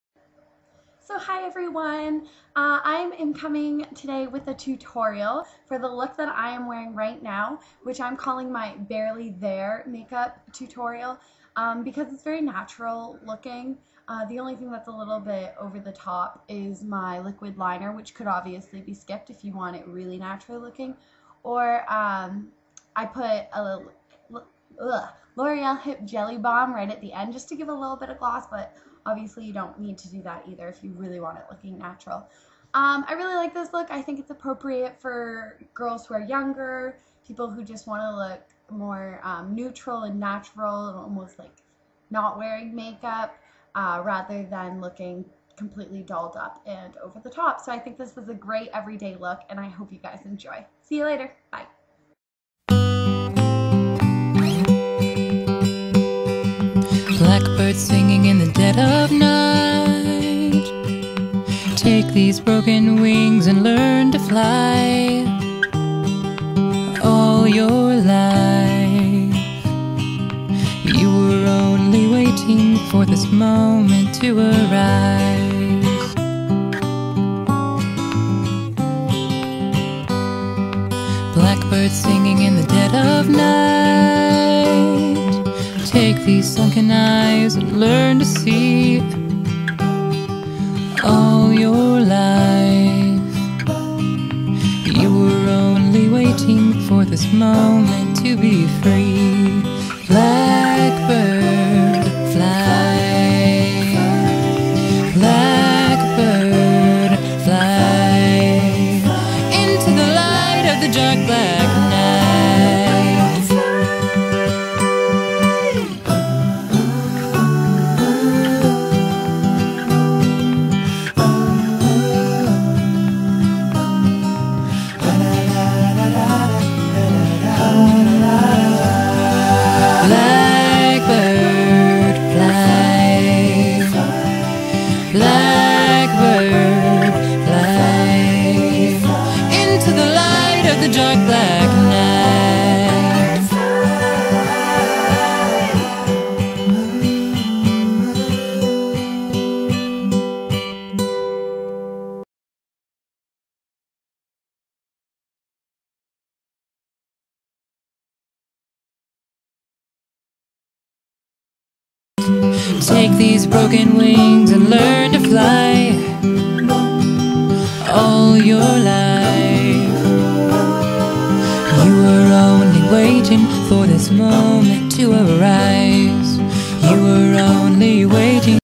Tutorial: Barely There
Hello everyone,

So here is what i am calling the "barely there" tutorial. This nice natural clean look is great whenever you want to look fresh and youthful.

Christie xo

Products Used:
Covergirl Natureluxe Silk Foundation in 310 "flax"
Benefit Stay Don't Stray
MAC Mineralized Skin Finish Natural in Medium
MAC Eyeshadow in Vanilla
Mally Pencil Eyeliner in Evershade
Annabelle Liquid Eyeliner in Espresso
E.l.f Lengthening Defining Mascara in Brown
Nars Blush in Orgasm
Benefit's Benetint
Loreal Hip Jelly Balm in Succulent